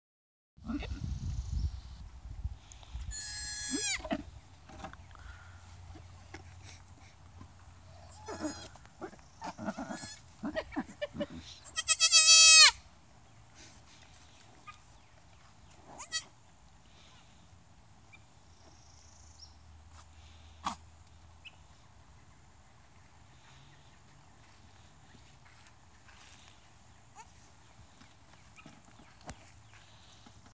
New Baby Goats Born Today 03-04-2018
1 baby boy, and 1 baby girl. AKA: kids.